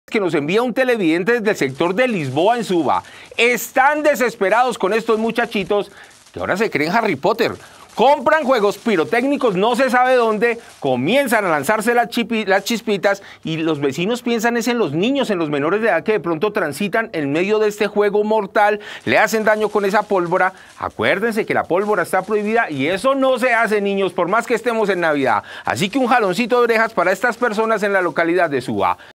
Peligroso juego con voladores entre jóvenes que se creen 'Harry Potter'
Se apuntan directamente al cuerpo con coloridos pirotécnicos, sin medir las consecuencias. No solo ellos podrían salir heridos, también ponen en riesgo a sus vecinos.

Ocurrió en la localidad de Suba, donde vecinos hacen un llamado al cuidado y al sentido común.